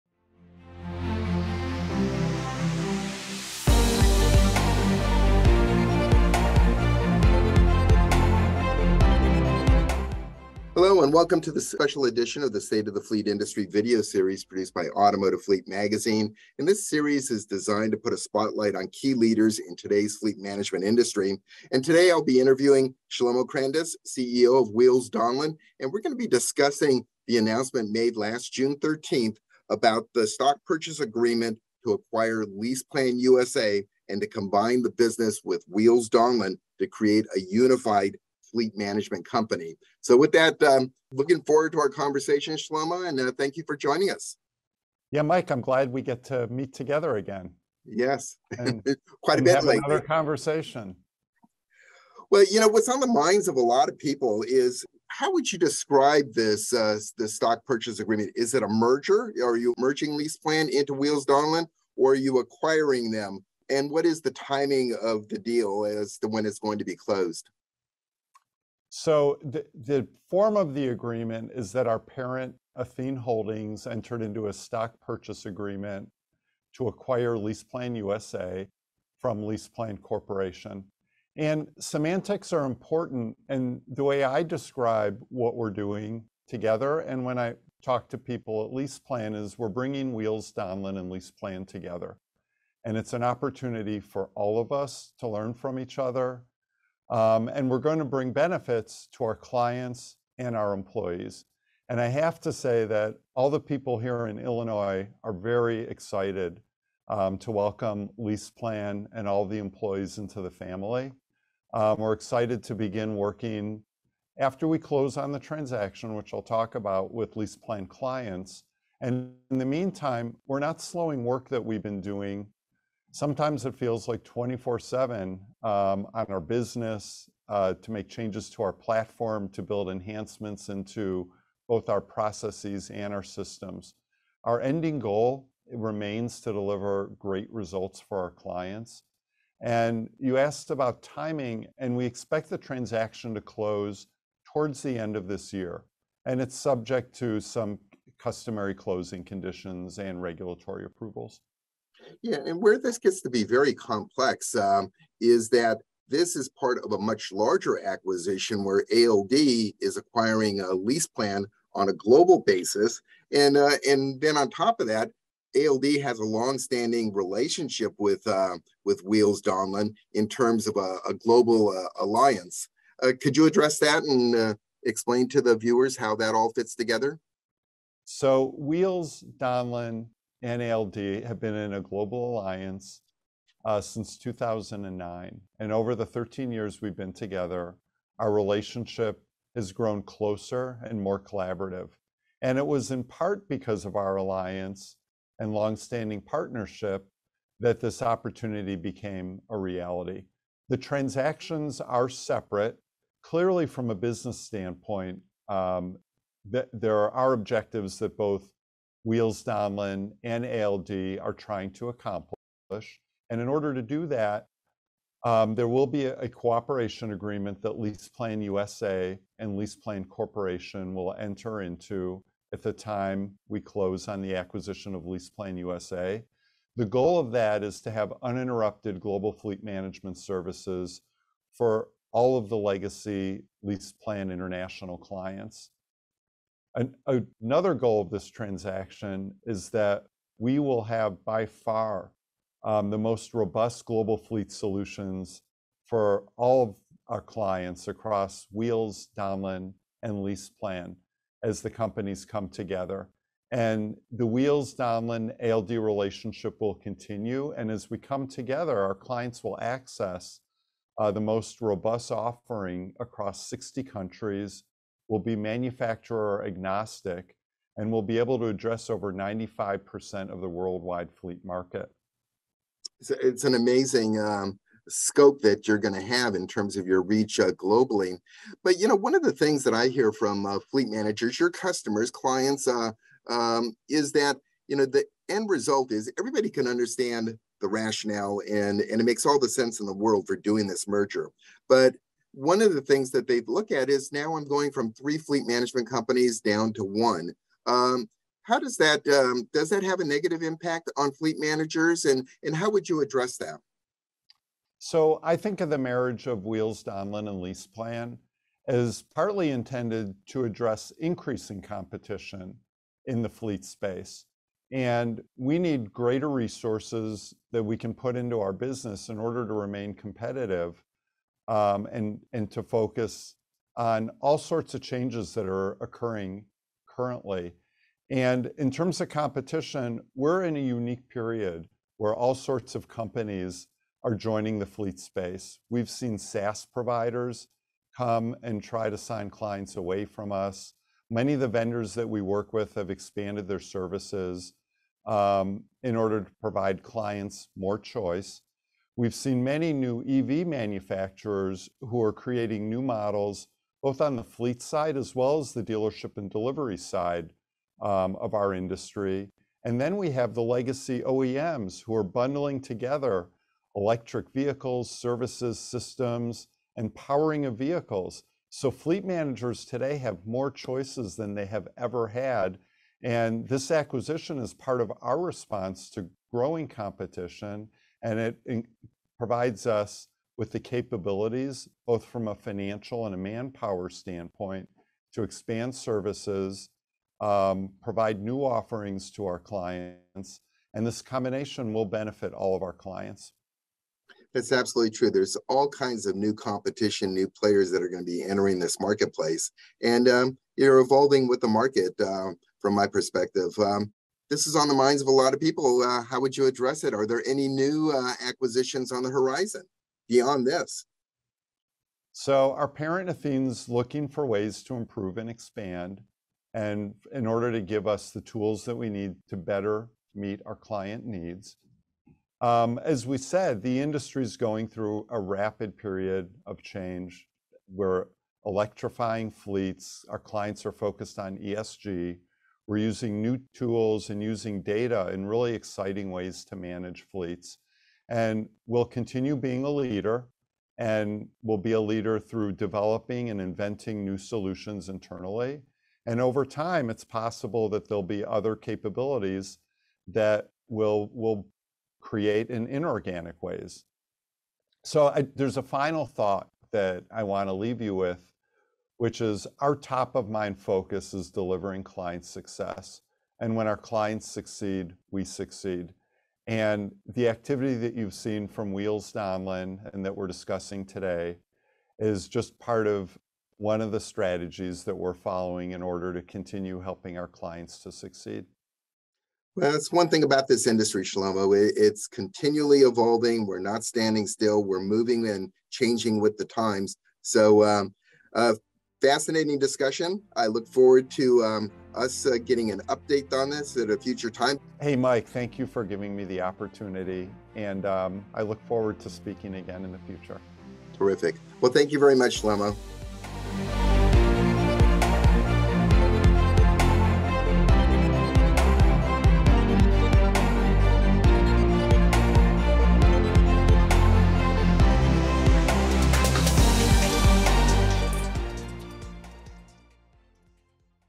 More Details on the Wheels Donlen Acquisition of LeasePlan USA
This State of the Fleet Industry Spotlight video produced by Automotive Fleet features an interview Wheels Donlen CEO Shlomo Crandus, who will discuss the June 13, 2022, announcement of the stock purchase agreement to acquire LeasePlan USA and combine the business with Wheels Donlen to create a unified fleet management company.

🎙 Today's topics include:
- When will the stock purchase agreement to acquire LeasePlan USA be closed?
- How does this agreement impact Wheels Donlen’s pre-existing relationship with ALD?
- How will this merger impact the fleet management industry?
- Are there other acquisitions on the horizon?

🤝 Follow and connect with Automotive Fleet on social media!

🎧 Prefer to listen?

⏱ Timestamps ⏱
0:00 Intro
1:08 Stock purchase agreement plans
2:30 Impacts on the relationship with ALD
4:33 Impact on the fleet management industry
6:40 Other acquisitions